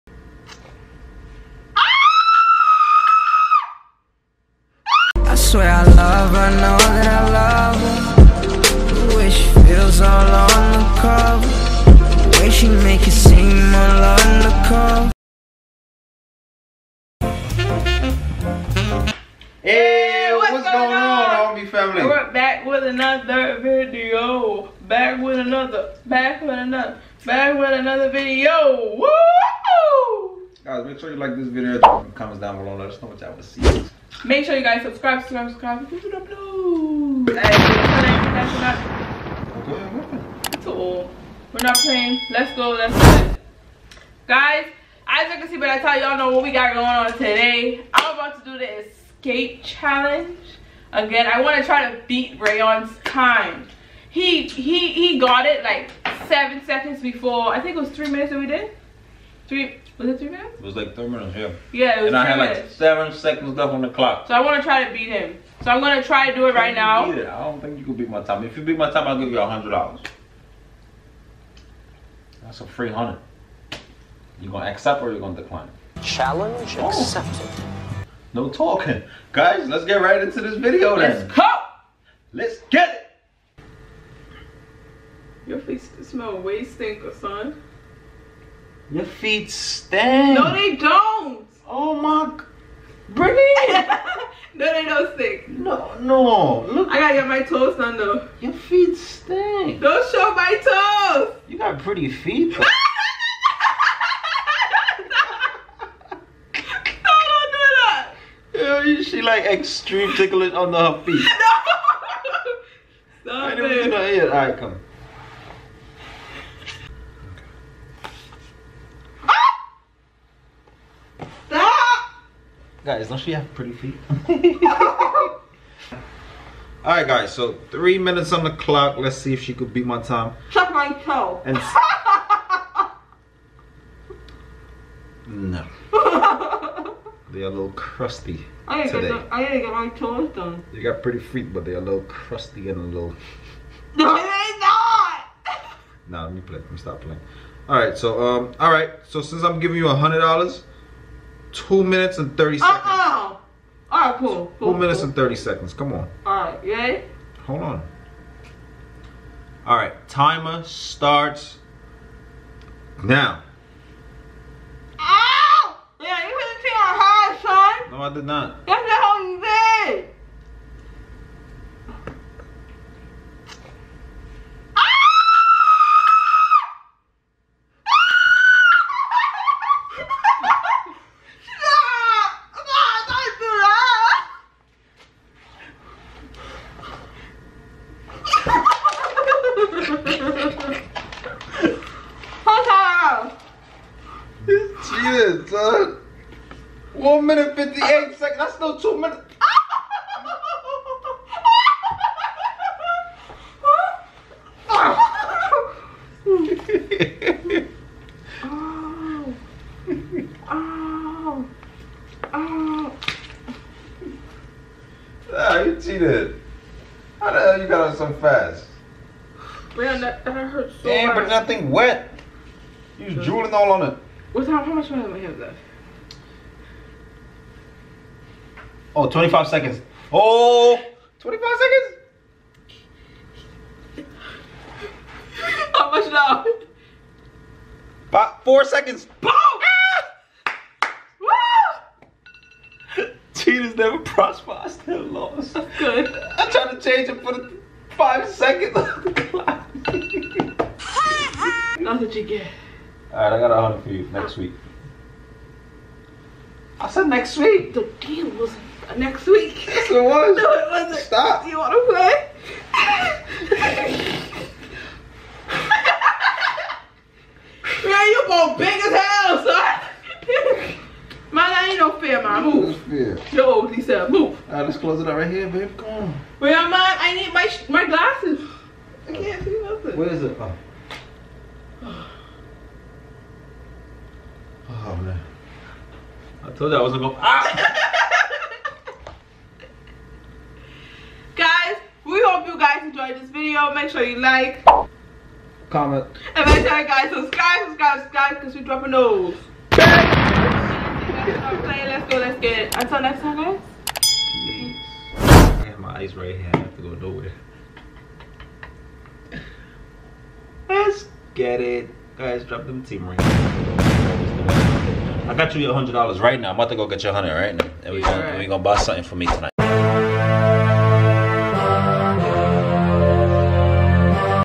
THE ESCAPE CHALLENGE BLINDFOLDED!! **Very Hard** | Rayon & Britney
Britney attempts the Escape challenge, and the winner walks away with $$$.

ESCAPE CHALLENGE BLINDFOLDED!! **Very Intense**

- PRODUCTS/VIDEO PROPS:
YOUTUBE PILLOW CASE
CAMERA USED
RING LIGHT
50inch TRIPOD
MINI TRIPOD